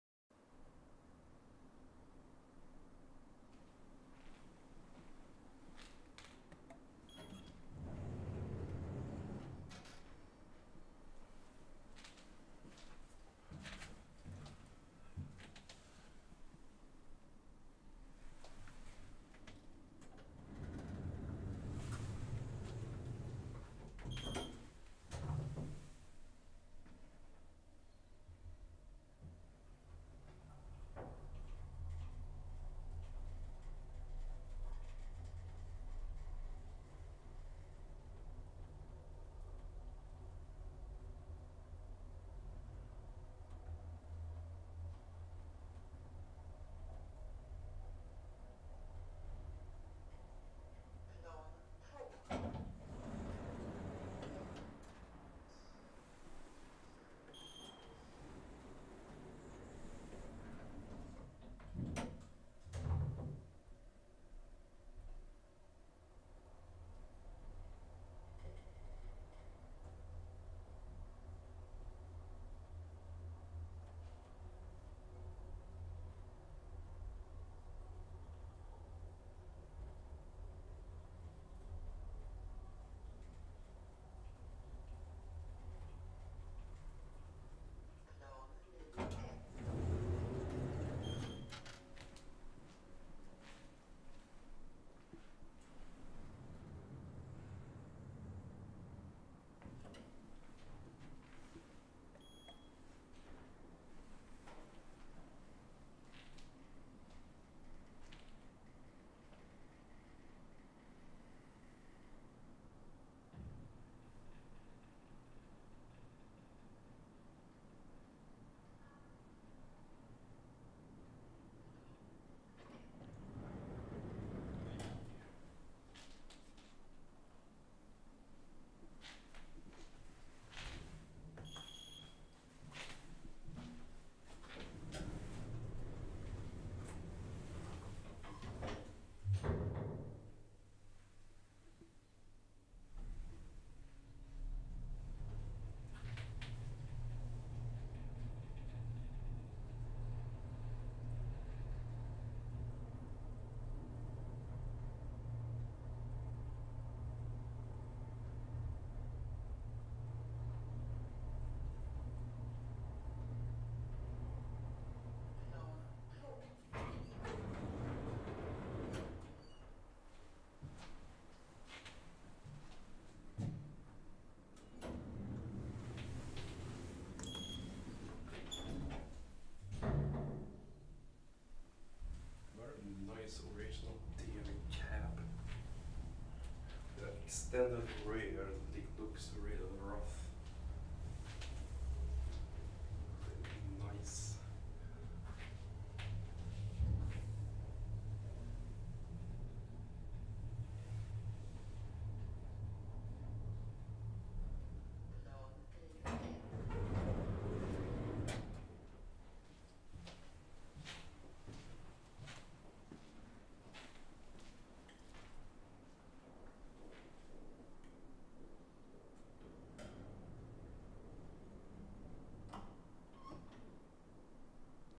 [LS] DEVE (MB: Trygga Hiss) Hydraulic Elevators @ Värmdövägen 195, Nacka, Sweden!
ELEVATOR INFORMATION!

Manufacturer/Tillverkare: DEVE
Model/Modell: TKID
Installation year/Byggår: ~1990
Current modernizer/Senaste modernisering av: Trygga Hiss AB
Latest modernization year/Senast moderniserat år:
Current model/Nuvarande modell: Hydroware HydroElite
Serial Number/Tillverkningsnummer: K-54573 & K-54572
Capacity/Maxvikt: 800Kg - 10 Persons / 1250Kg - 15 Persons
Floors/Antal våningar: 4 = -1 0 1 2

Location/Adress: Värmdövägen 195, 131 40 Nacka (Nacka Forum) SWEDEN!
Camera/Kamera: Sony XPERIA 1 IV